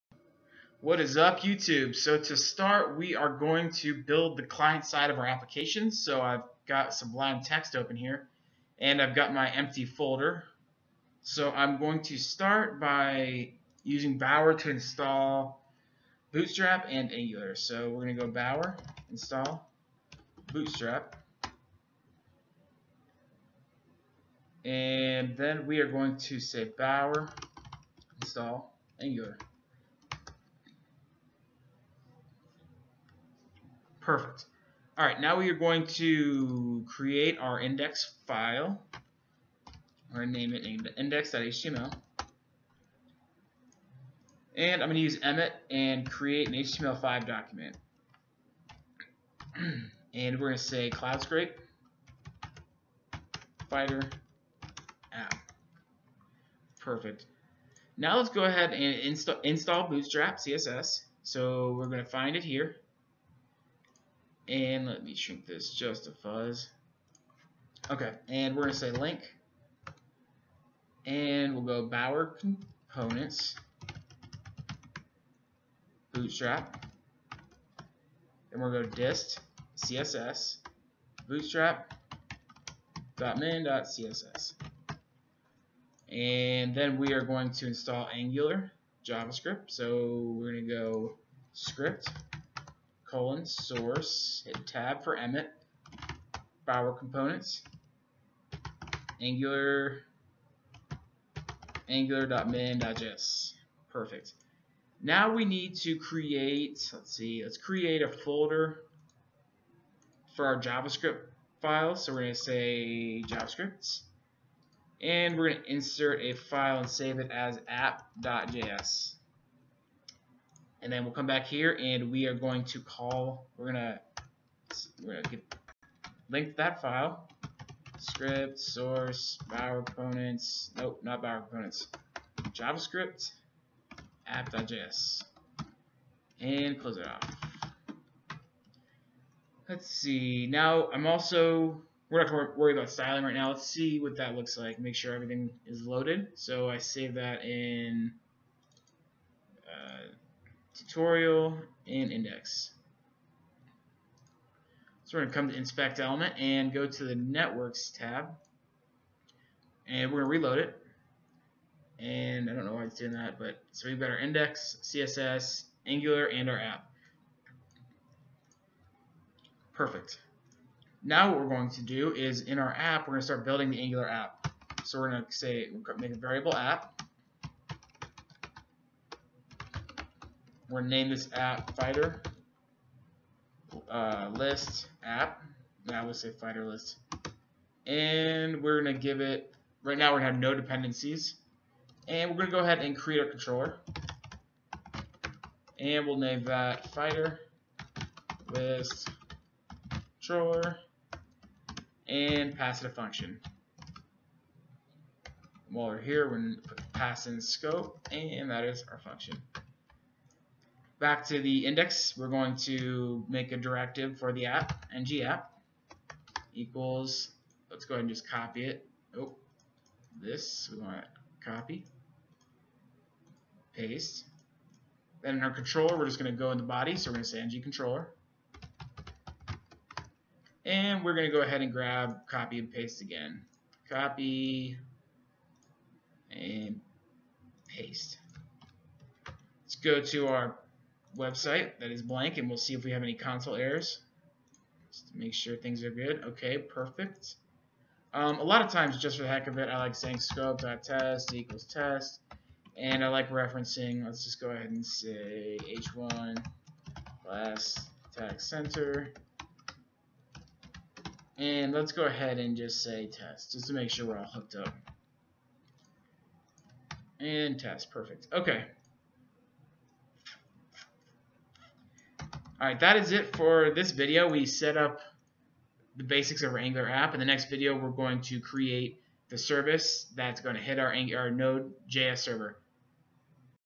Setting Up Angular for Web Scrapping App Part 1
Setting Up Angular for Web Scrapping App using CloudScrape for our data. MEAN stack application. We are building a fighter directory that is updated daily with CloudScrape.